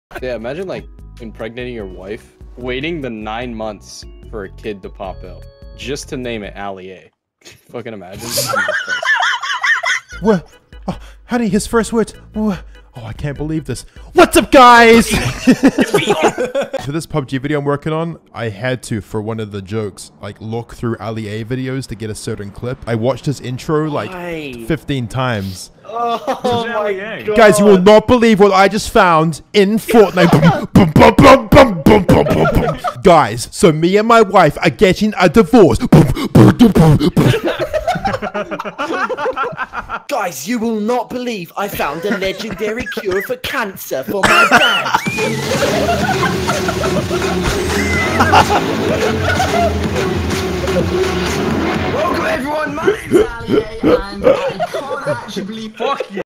MAKING FUN OF Ali A| Best of Fitz
This is a funny scene of a video of Fitz.

- - - LINKS - - -

- - - INFORMATION - - -

If you (Fitz, ...) want me to delete this video please tell me!